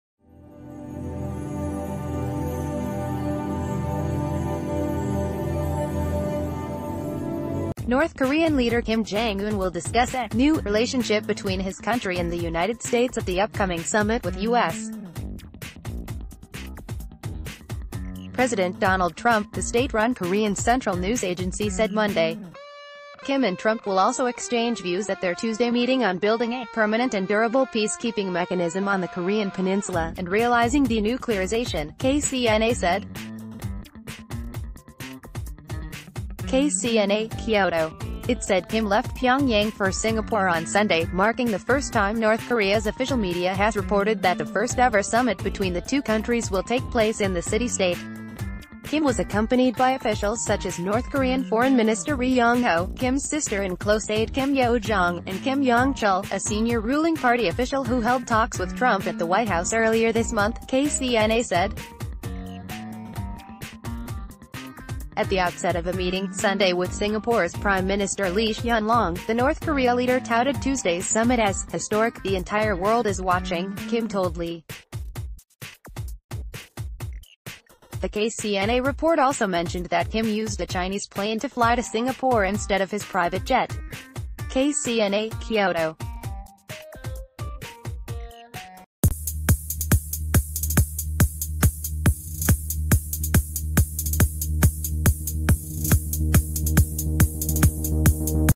Breaking News | Kim, Trump to discuss "new" N. Korea-U.S. relations at summit: KCNA
North Korean leader Kim Jong Un will discuss a "new" relationship between his country and the United States at the upcoming summit with U.S. President Donald Trump, the state-run Korean Central News Agency said Monday. Kim and Trump will also exchange views at their Tuesday meeting on building a "permanent and durable peace-keeping mechanism on the Korean Peninsula" and realizing denuclearization, KCNA said. [KCNA/Kyodo] It said Kim left Pyongyang for Singapore on Sunday, marking the first time ...